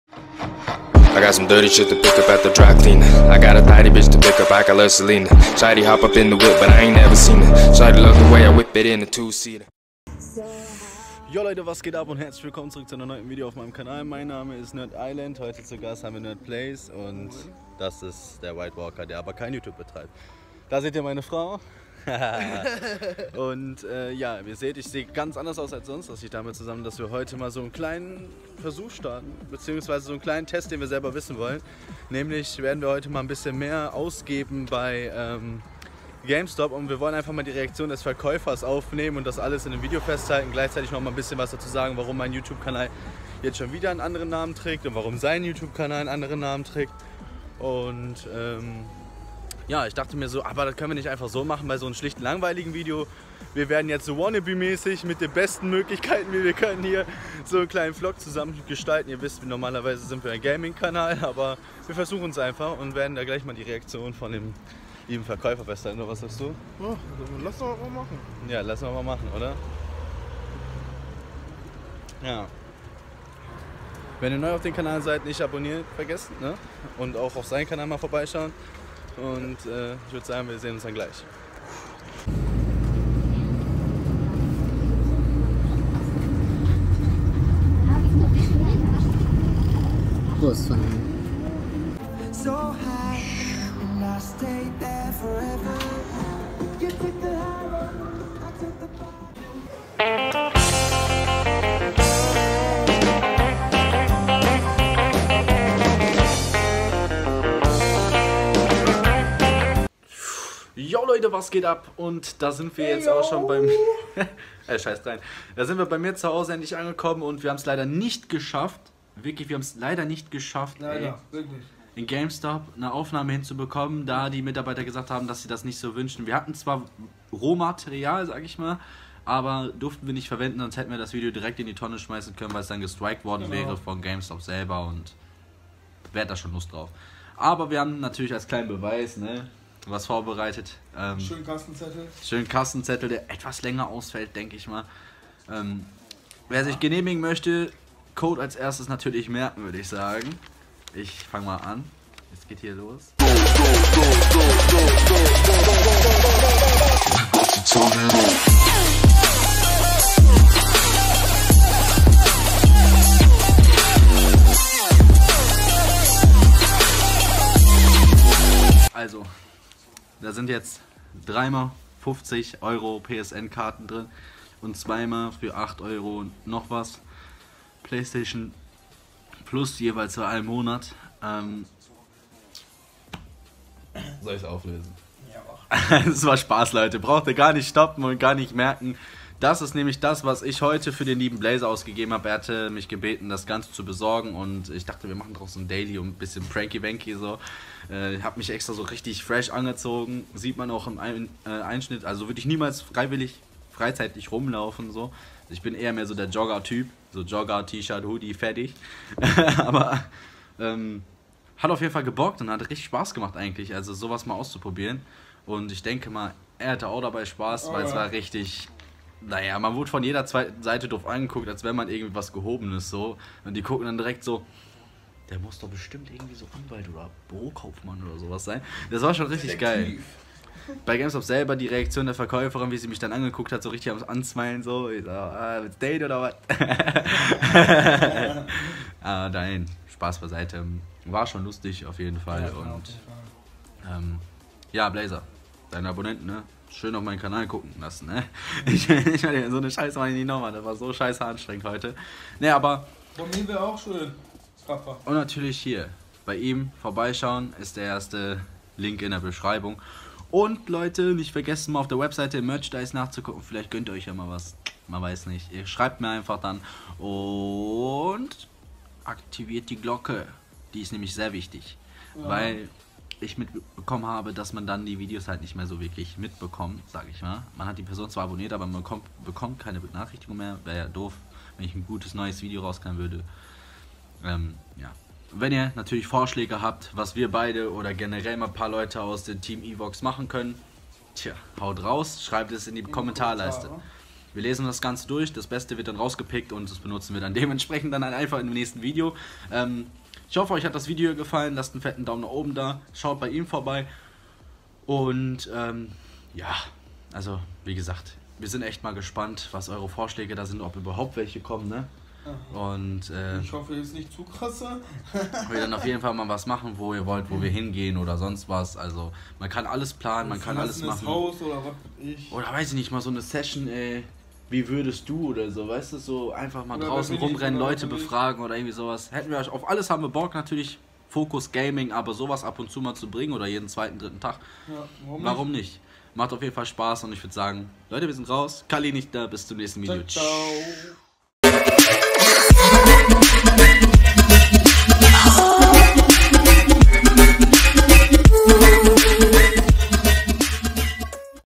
Auf nach GAMESTOP vlog ✅ |Nerdisland
Öffnen ¥

peace NERDS  was geht euer NERDISLAND hier Heute erwartet euch ein Game stop Vlog einer versuchten anderen Art  (SORRY FÜR DIE MIESSSEEEEE QUALLI) natürlich bitte nimm darauf Rücksicht dass ich natürlich kein Profi Vlogger bin aber wir hatten mal Lust darauf naja es hat halbwegs hin gehauen und ich hoffe natürlich das es euch gefallen hat wir sehen uns dann im nächsten Video Peace out ! Natürlich wenn mehr dann gucken wir das die Qualli viel besser wird